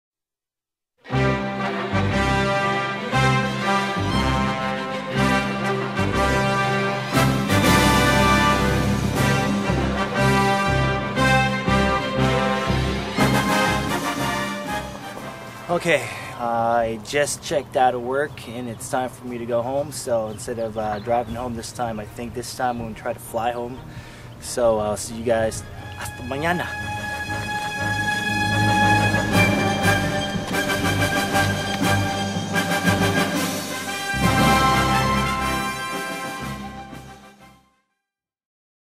Super BART Train Operator
This is my very first attempt at flying. I still need to perfect the art.